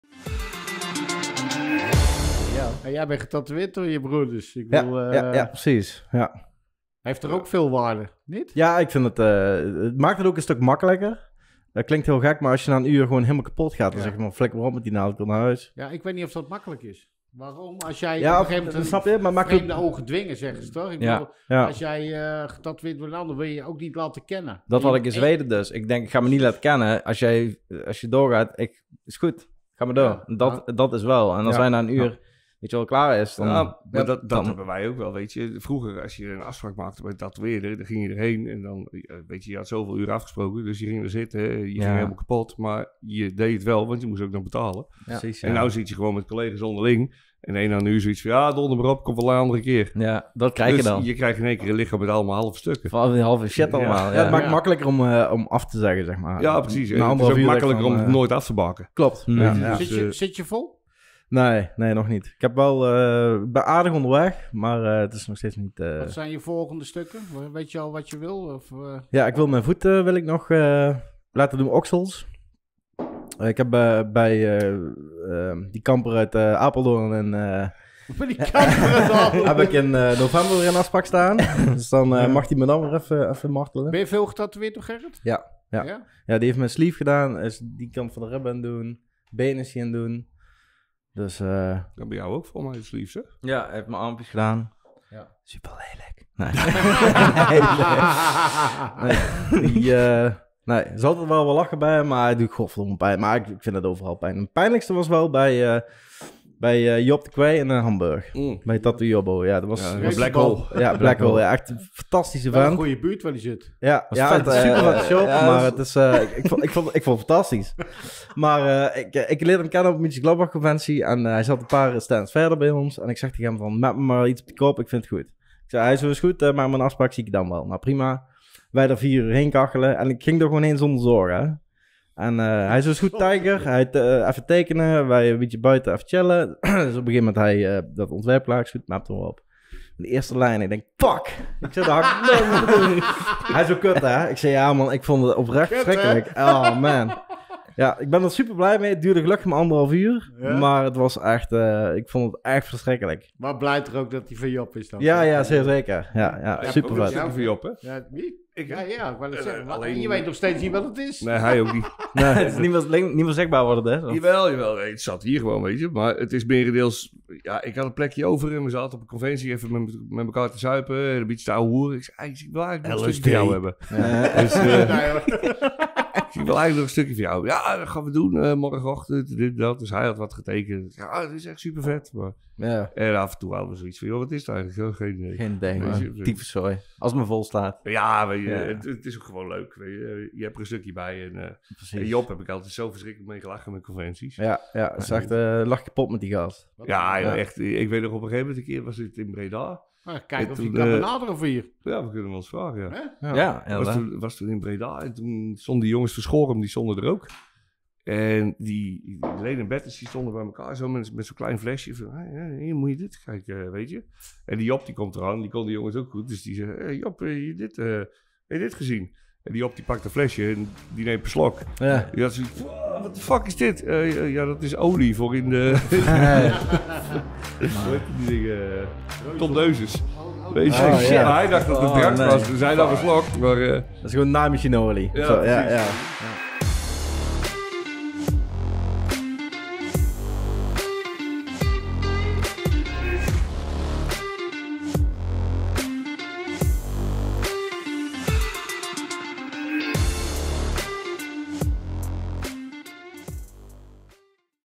Jeroen Janssen vertelt over zijn tattoo en Job drinkt naaimachine olie | Inkt Smijters Podcast
Jeroen Janssen vertelt over het zetten van zijn tattoo en Job drinkt naaimachine olie. Een kort fragment uit onze podcast met de gebroeders Janssen.